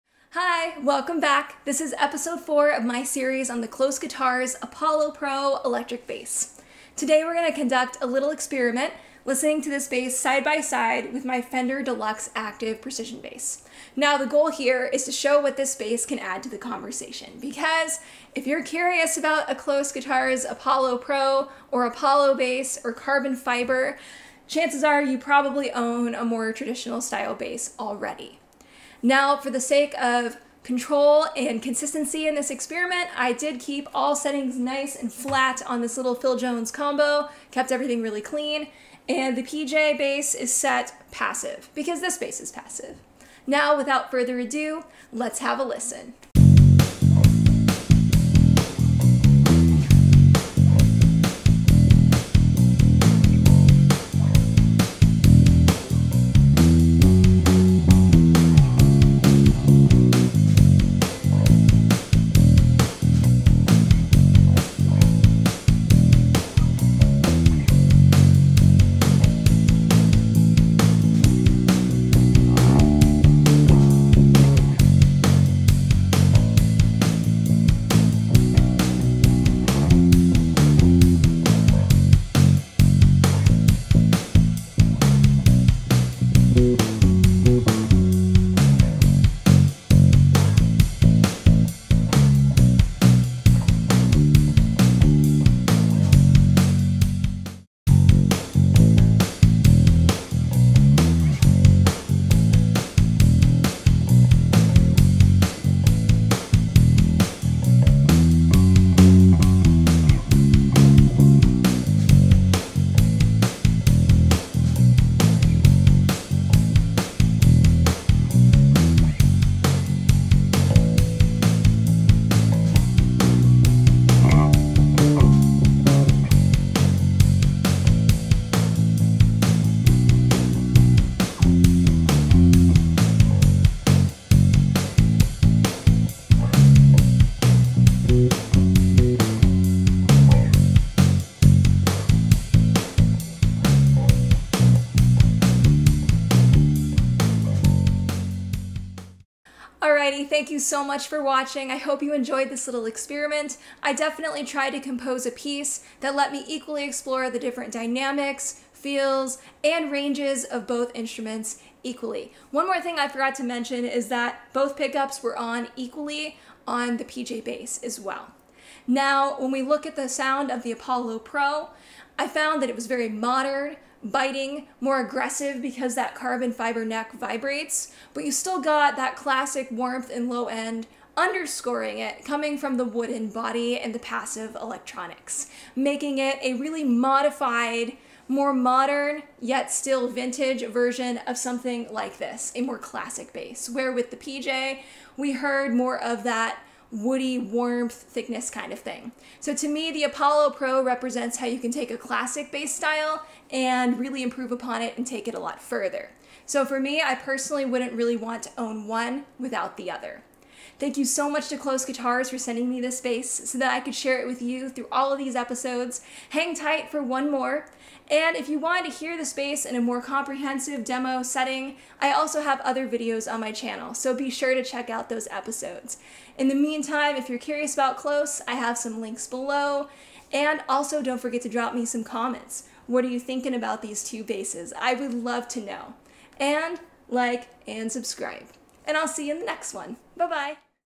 KLOS Apollo Pro Bass vs. Precision Bass
Episode 4: This is the KLŌS Apollo Pro Bass compared to my Fender Deluxe Active Precision Bass. The goal is to add to the conversation on how the carbon fiber Apollo Pro fits into the world of classic electric basses. Enjoy!

0:00 - INTRO
0:46 - SAMPLE: KLŌS Apollo Pro Bass
1:38 - SAMPLE: Fender P Bass
2:30 - MY THOUGHTS
3:31 - THANK YOU & DISCLOSURE

SHOP OTHER GEAR USED IN THIS VIDEO (from Sweetwater):
- Fender Player Plus Active Precision Bass (similar to this Deluxe Active)